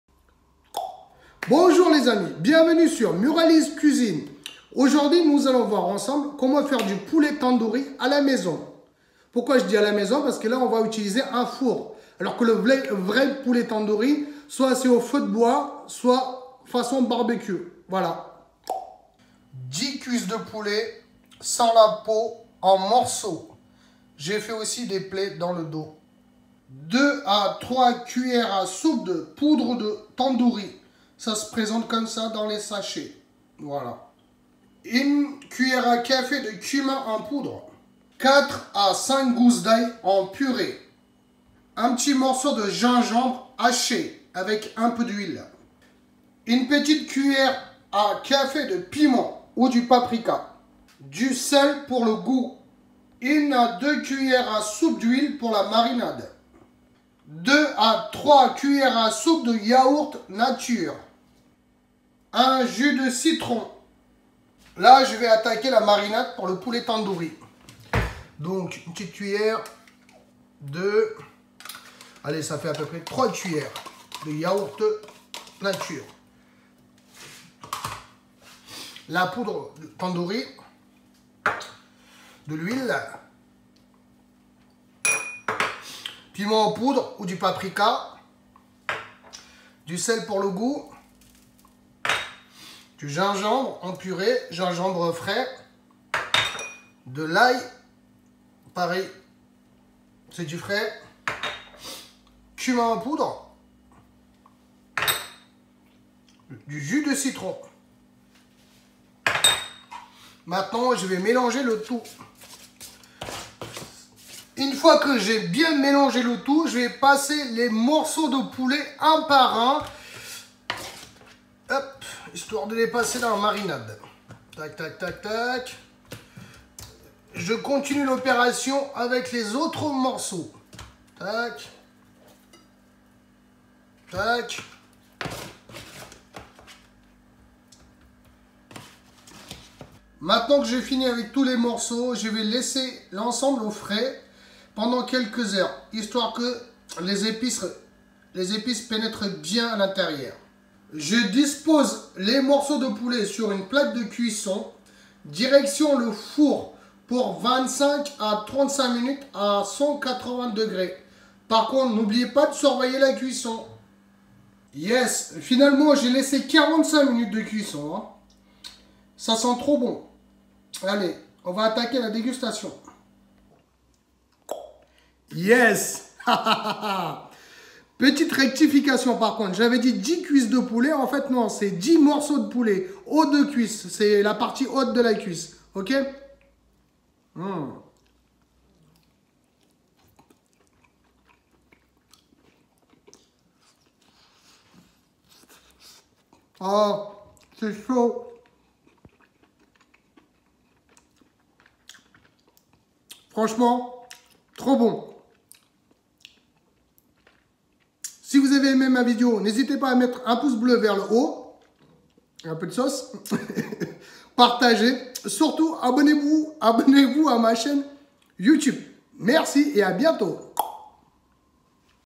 POULET TANDOORI / TANDOORI CHICKEN
10 haut de cuisses /ou 5 cuisses de poulet
3 C à S d’épices Tandoori
1 C à C de cumin en poudre
5 gousses d’ail en purée
Gingembre en purée
1 C à C de piment ou du paprika
Sel
2 C à C d’huile
1 yourte nature
1 jus de citron
Prévoir 45 min de cuisson